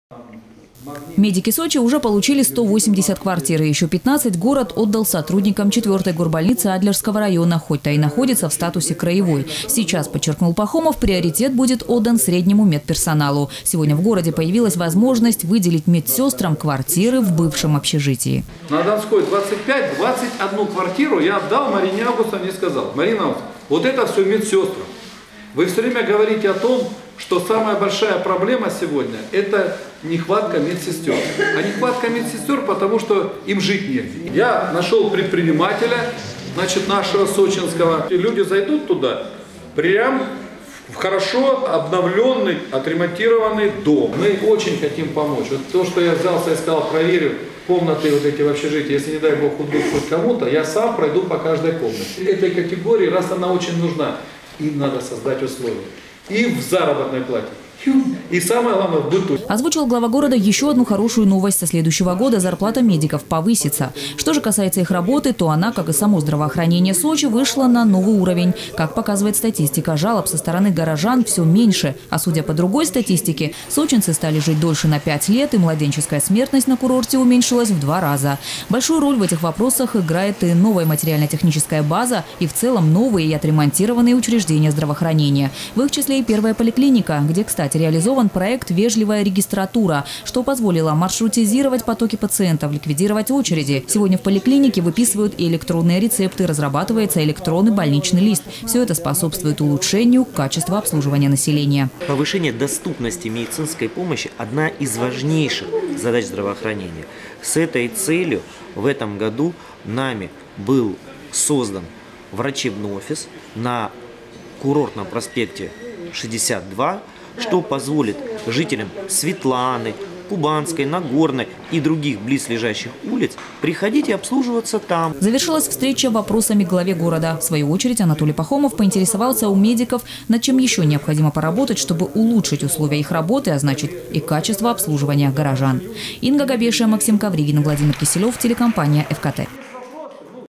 Сочинским медсестрам вручат ключи от квартир. Новости Эфкате Сочи
Об этом заявил глава города Анатолий Пахомов на традиционной встрече с медиками, посвященной подведению итогов работы здравоохранения в 2017-ом году.

Основными нововведениями уходящего года стал проект «Вежливая регистратура» и введение в поликлиниках электронных рецептов. Медики Сочи уже получили 180 квартир. И ещё 15 город отдал сотрудникам 4-ой горбольницы Адлерского района, хоть та и находится в статусе краевой. Сейчас, подчеркнул Пахомов, приоритет будет отдан среднему медперсоналу. Сегодня в городе появилась возможность выделить медсестрам квартиры в бывшем общежитии.

Сочи. Новости.
Телеканал Эфкате.